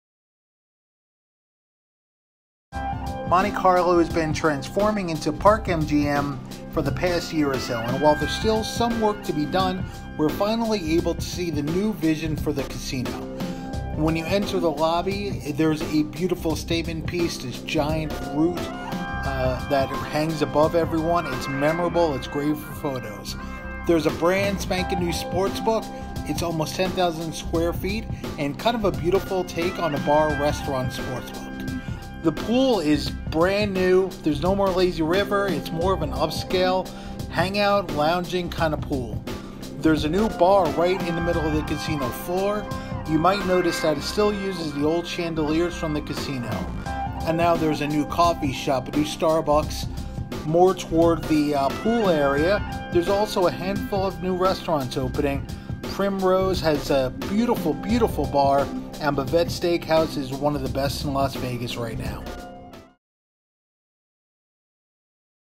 Listing Las Vegas: Top 5 New Venues At Park MGM
The Monte Carlo has slowly been transformed in the Park MGM over the past year, and Marc Meltzer takes you on a tour of the revamped property just across from T-Mobile Arena, including the latest new sportsbook on the Las Vegas Strip.